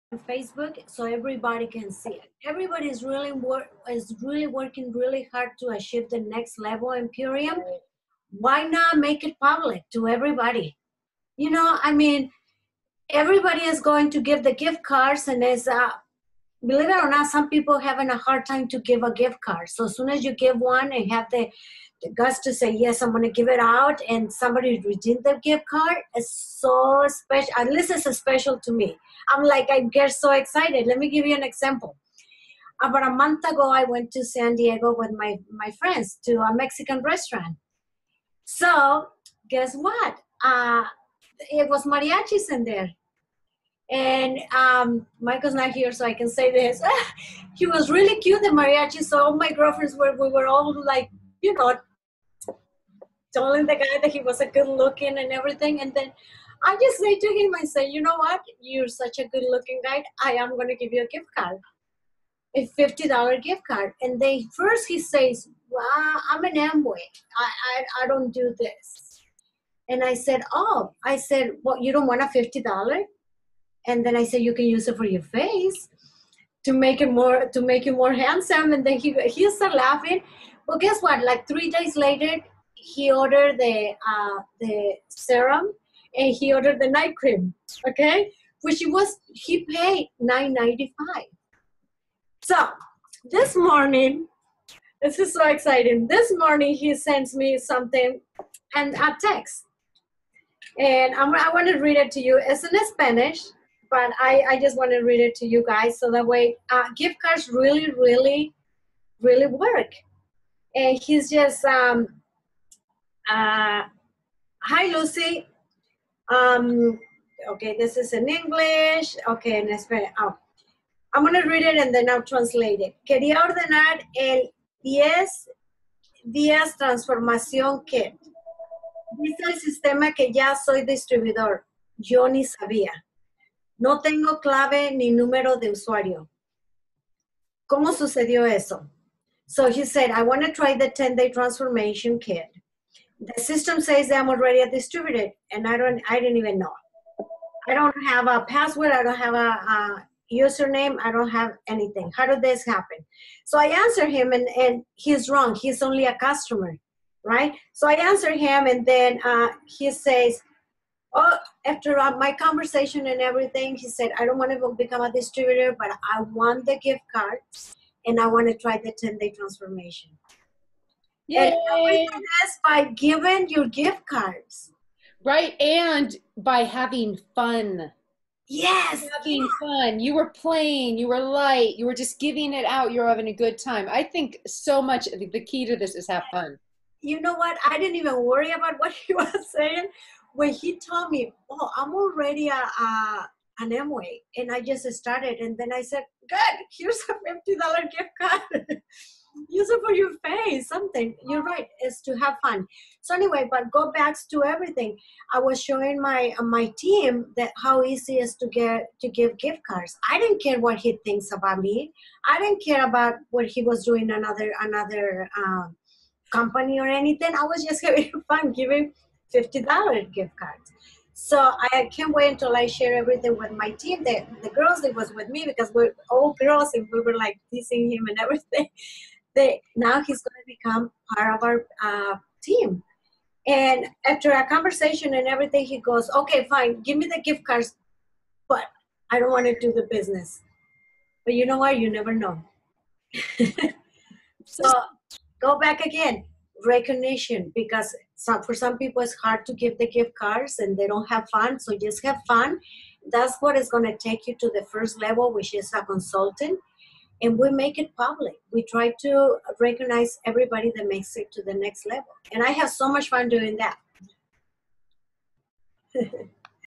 Gift Card Marketing with Lucy Mazes
Lucy Mazes share a success story... giving out a Gift Card.